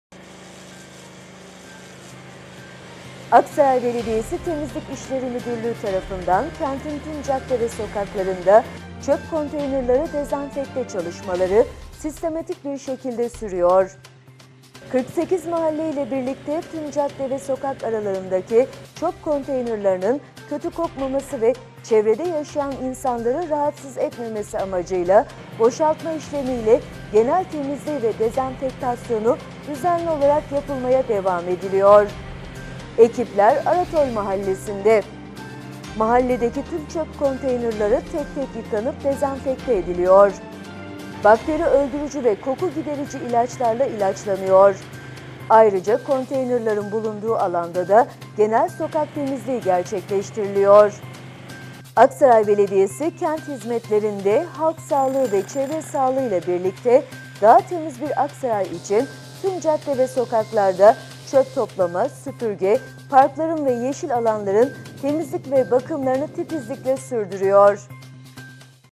ARATOL MAHALLESİ’NDE TÜM ÇÖP KONTEYNERLERİ TEK TEK YIKANIP DEZENFEKTE EDİLİYOR
AKSARAY BELEDİYESİ TEMİZLİK İŞLERİ MÜDÜRLÜĞÜ TARAFINDAN KENTİN TÜM CADDE VE SOKAKLARINDA ÇÖP KONTEYNERLERİ DEZENFEKTE ÇALIŞMALARI SİSTEMATİK BİR ŞEKİLDE SÜRÜYOR.
48 MAHALLE İLE BİRLİKTE TÜM CADDE VE SOKAK ARALARINDAKİ ÇÖP KONTEYNERLERİNİN KÖTÜ KOKMAMASI VE ÇEVREDE YAŞAYAN İNSANLARI RAHATSIZ ETMEMESİ AMACIYLA BOŞALTMA İŞLEMİYLE GENEL TEMİZLİĞİ VE DEZENFEKTASYONU DÜZENLİ OLARAK YAPILMAYA DEVAM EDİLİYOR.
EKİPLER, ARATOL MAHALLESİ’NDE.
MAHALLEDEKİ TÜM ÇÖP KONTEYNERLERİ TEK TEK YIKANIP DEZENFEKTE EDİLİYOR.  BAKTERİ ÖLDÜRÜCÜ VE KOKU GİDERİCİ İLAÇLARLA İLAÇLANIYOR. AYRICA KONTEYNERLERİN BULUNDUĞU ALANDA DA GENEL SOKAK TEMİZLİĞİ GERÇEKLEŞTİRİLİYOR.
AKSARAY BELEDİYESİ KENT HİZMETLERİNDE HALK SAĞLIĞI VE ÇEVRE SAĞLIĞI İLE BİRLİKTE DAHA TEMİZ BİR AKSARAY İÇİN TÜM CADDE VE SOKAKLARDA ÇÖP TOPLAMA, SÜPÜRGE, PARKLARIN VE YEŞİL ALANLARIN TEMİZLİK VE BAKIMLARINI TİTİZLİKLE SÜRDÜRÜYOR.